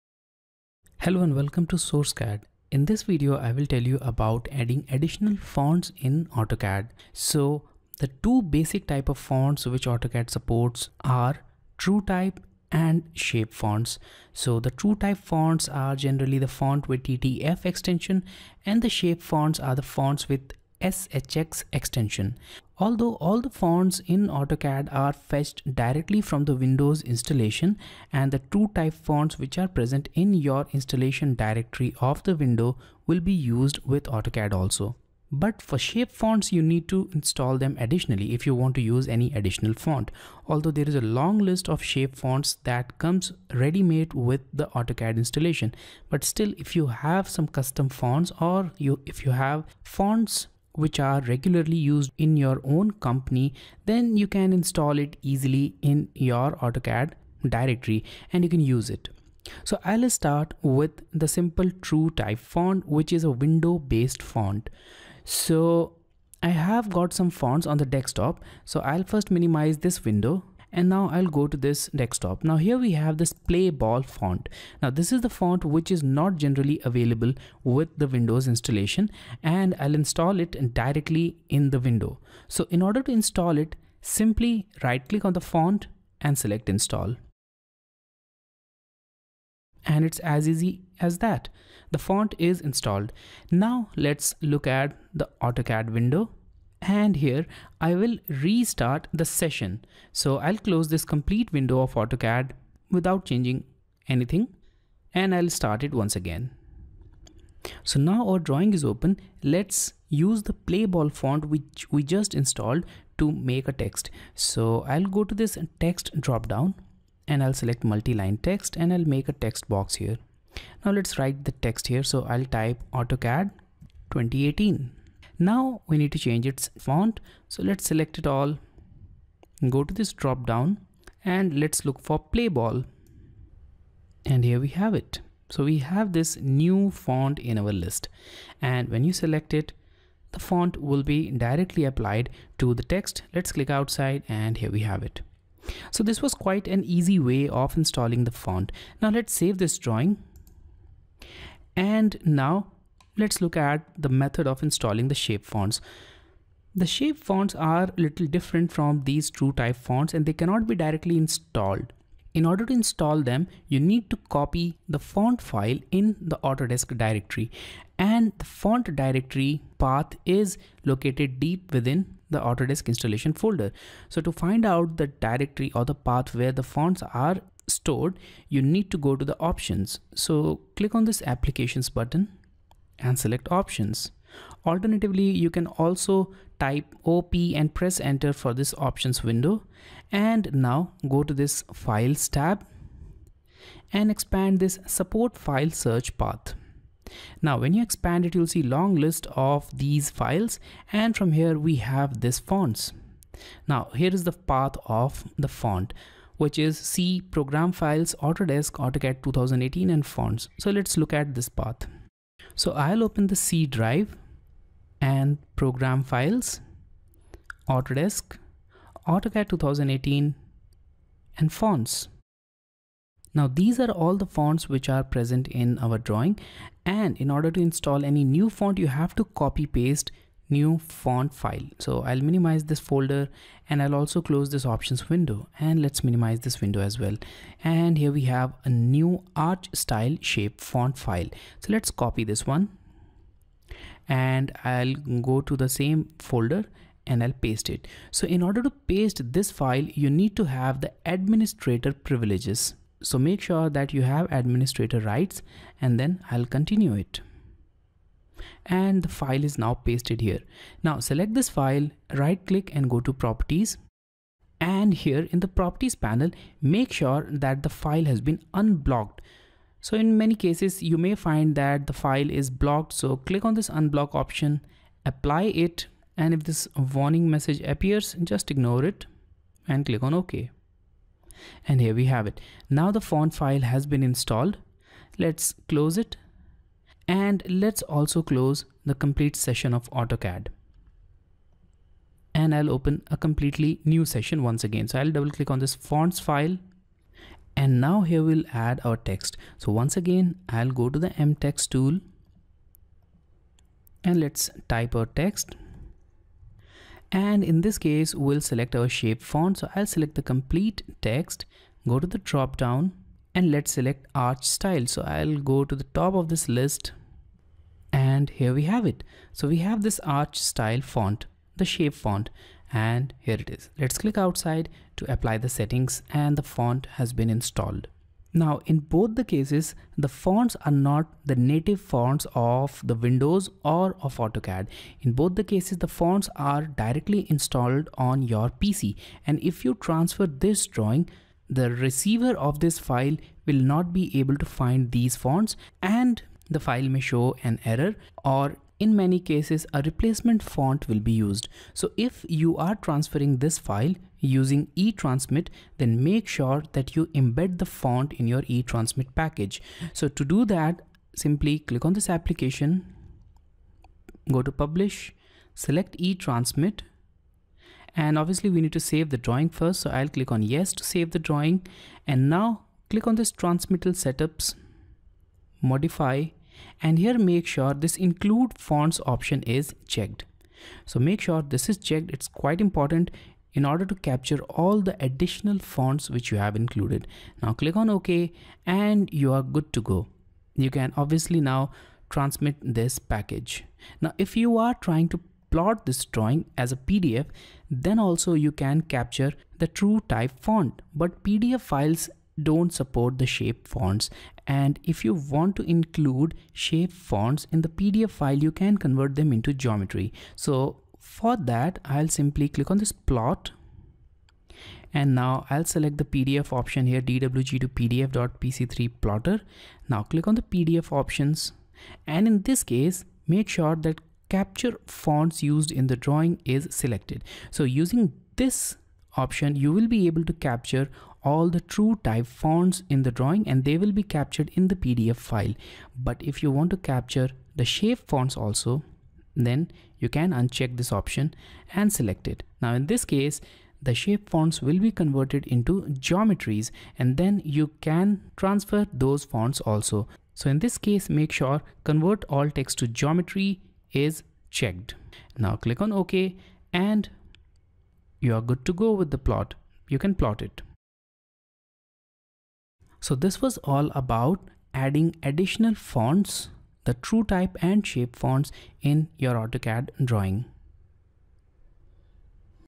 How to add additional fonts in AutoCAD
Learn AutoCAD right from scratch with full-length courses, instructor support and certificate all for one Subscription

In this video, I have explained methods of adding additional TrueType and shape fonts in AutoCAD.
You can download fonts used in this drawing from the link mentioned here.